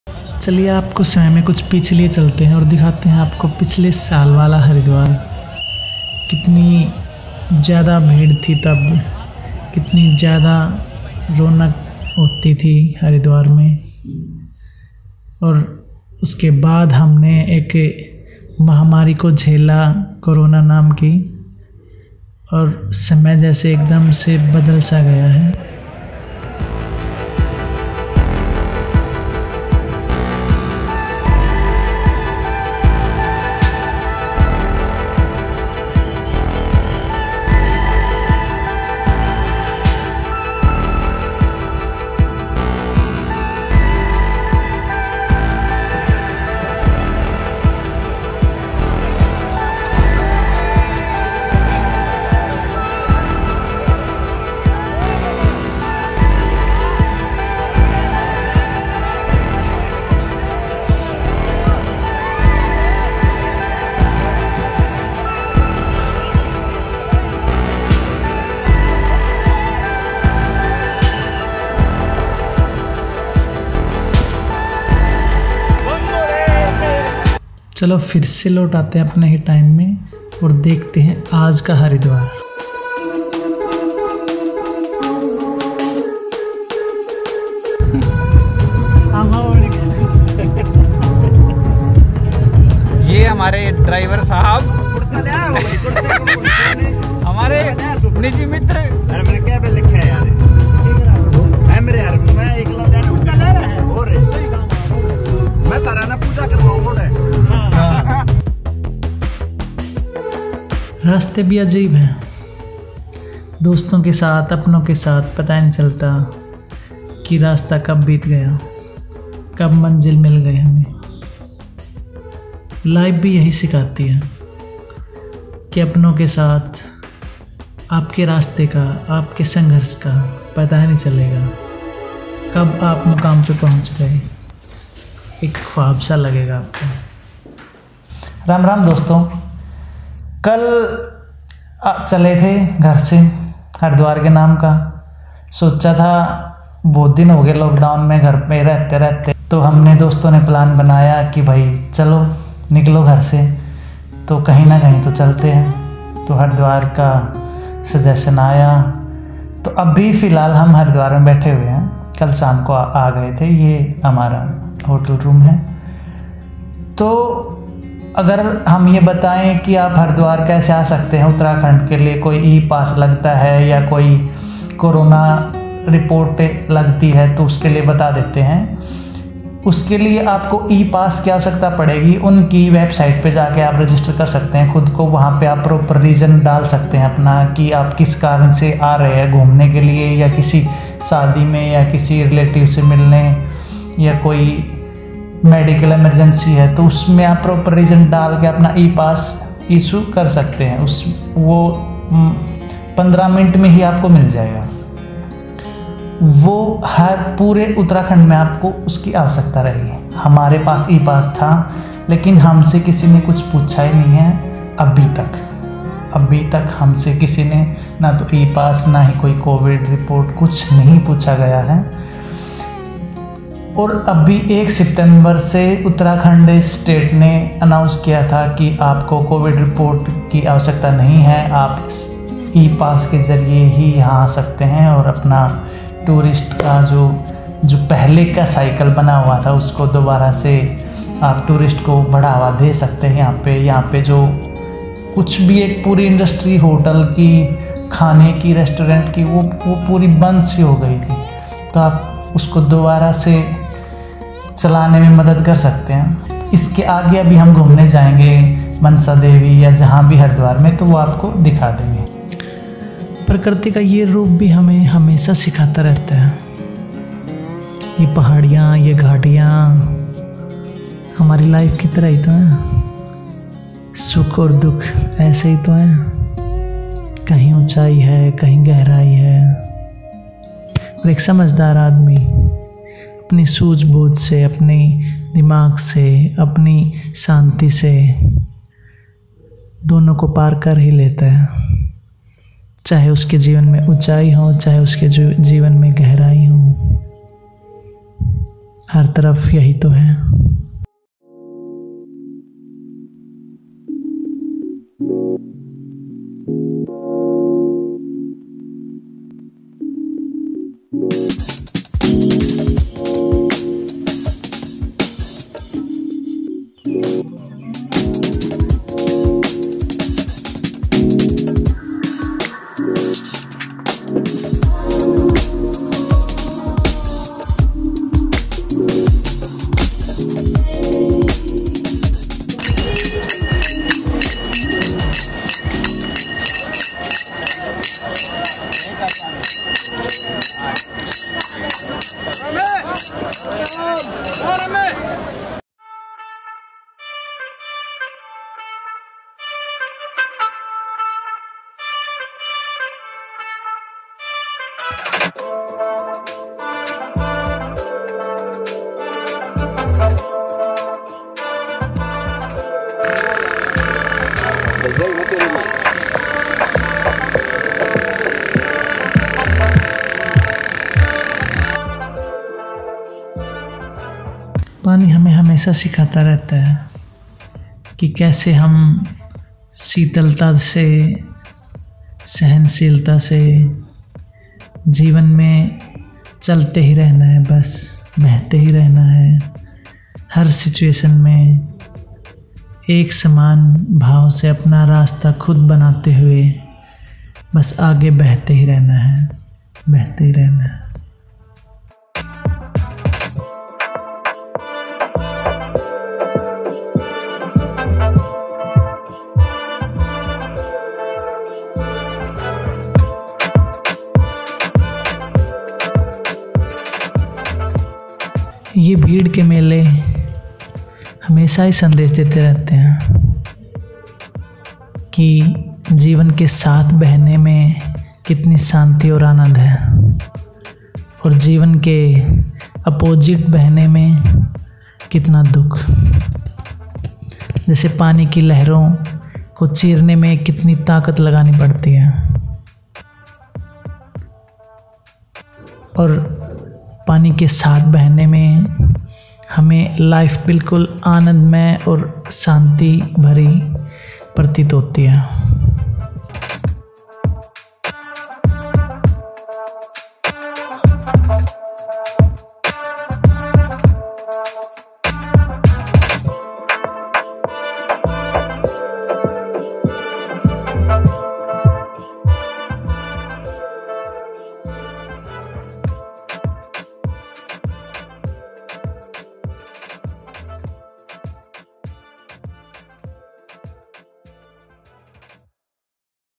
Haridwar after Lockdown | e-pass or COVID-Report | Tourism after Lockdown
Hariyana to Haridwar trip after Lockdown | e-pass or COVID-Report | Tourist after Lockdown   SwaNirmaan
In this video, we explain do we need any COVID-19 negative report or any other documents to visit in Haridwar.

- Manjeet Lakra
Software Engineer
Founder of Kabira WEB (Website Development Company)